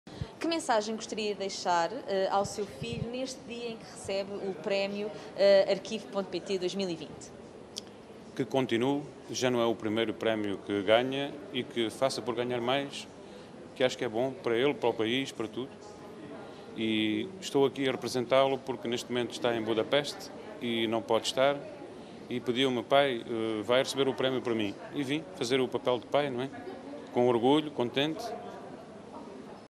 Testemunho do pai de Miguel Ramalho, 1º classificado do Prémio Arquivo.pt 2020
Miguel Ramalho, a trabalhar no estrangeiro, autor do trabalho premiado "Desarquivo", foi representado pelo pai no dia da entrega do Prémio Arquivo.pt 2020. Este foi o seu testemunho.